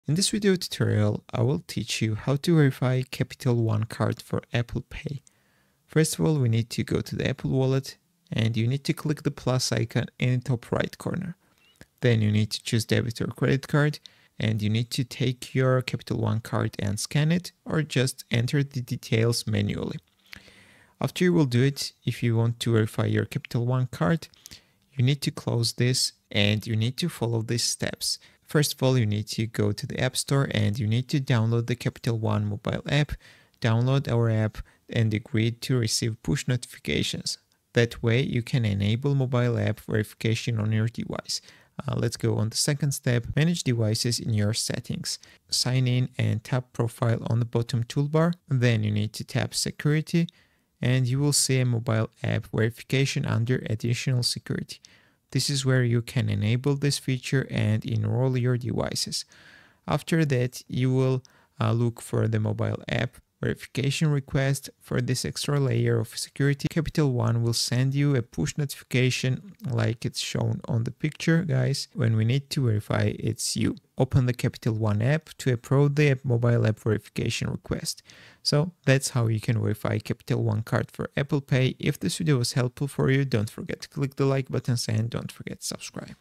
How To Verify Capital One Card For Apple Pay 2025
How To Verify Capital One Card For Apple Pay

In this video, I’ll show you how to verify your Capital One card for Apple Pay.